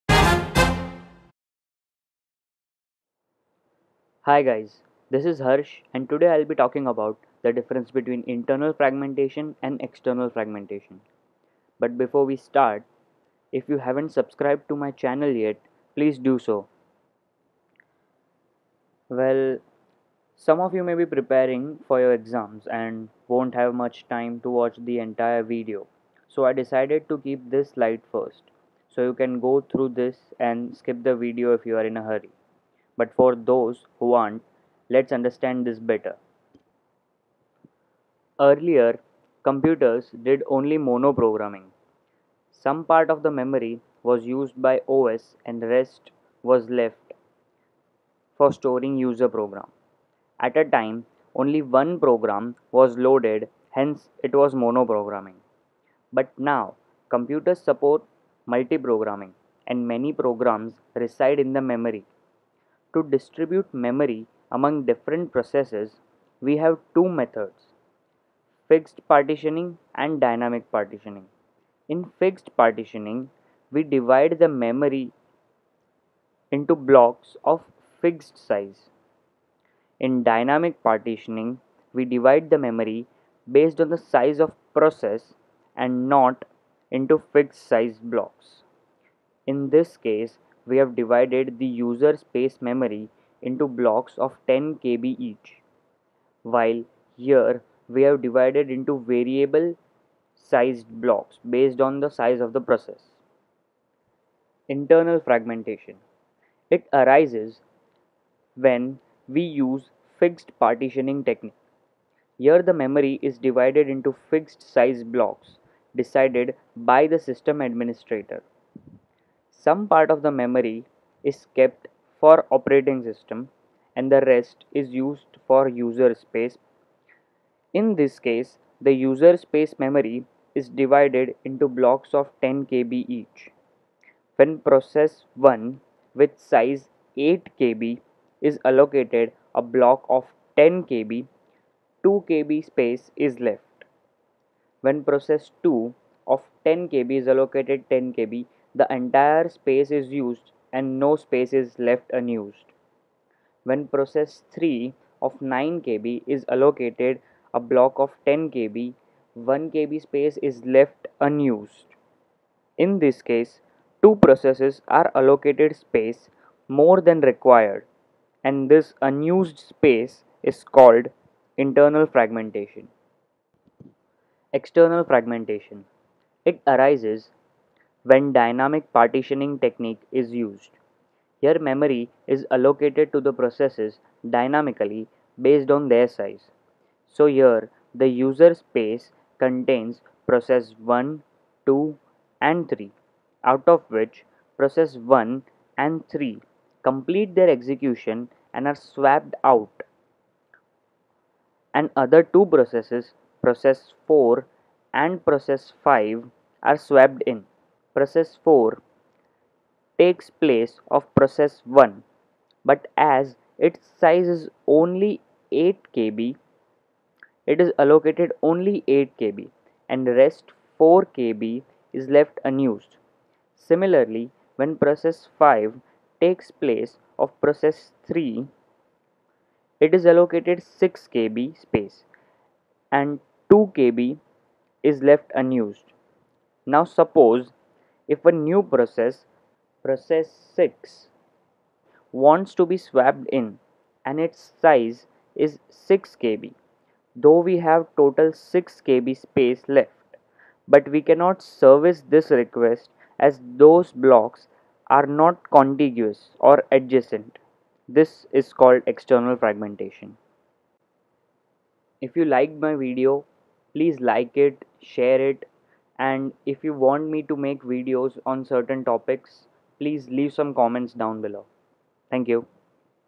Difference between internal and external fragmentation in operating system
In this video i will be explaining the difference between internal and external fragmentation or two types of fragmentation based on memory management techniques.

Internal Fragmentation

1)When a process is allocated more memory than required, few space is left unused and this is called as INTERNAL FRAGMENTATION.
2)It occurs when memory is divided into fixed-sized partitions.
3) It can be cured by allocating memory dynamically or having
partitions of different sizes.

External Fragmentation

1)After execution of processes when they are swapped out of memory and other smaller processes replace them, many small non contiguous(adjacent) blocks of unused spaces are formed which can serve a new request if all of them are put together but as they are not adjacent to each other a new request can't be served and this is known as EXTERNAL FRAGMENTATION.
2)It occurs when memory is divided into variable-sized partitions based on size of process.
3)It can be cured by Compaction, Paging and Segmentation.

Internal Fragmentation

1)It arises when we use fixed sized partitioning.
2)Some part of the memory is kept for operating system and the rest is available for user space.
3)In this case the user space is divided into blocks of 10 KB each.
When process 1 with size 8 KB is allocated a block of 10 KB, 2 KB space is left unused. When process 2 with size 10 KB is allocated a !0 KB block no space is left unused. When process 3 with size 9KB is allocated a 10 KB block, 1KB is left unused.
4)Here 2 processes are allocated space more than required and this unused space is so small to store a new process and is wasted. This is called as INTERNAL FRAGMENTATION.

External Fragmentation

1)It arises when dynamic partitioning technique is used.
2)Here memory is allocated to the processes dynamically based on their size.
3)So in the above example, the user space contains processes 1,2 & 3 Out of which process 1 & 3 complete their execution and are swapped out and two other processes, process 4 & 5 are swapped in their places.
4)Process 4 takes place of process 1 but as its size is only 8 KB, it is allocated only 8 KB and rest is left unused.
Process 5 takes place of process 3. It is allocated 6 KB space and 8 KB is left unused.
5)Now suppose a new process, process 6 wants to be swapped in and its size is 6 KB. Though we have total 6 KB space but we cannot service this request as these blocks are not contiguous(adjacent). This is called as EXTERNAL FRAGMENTATION.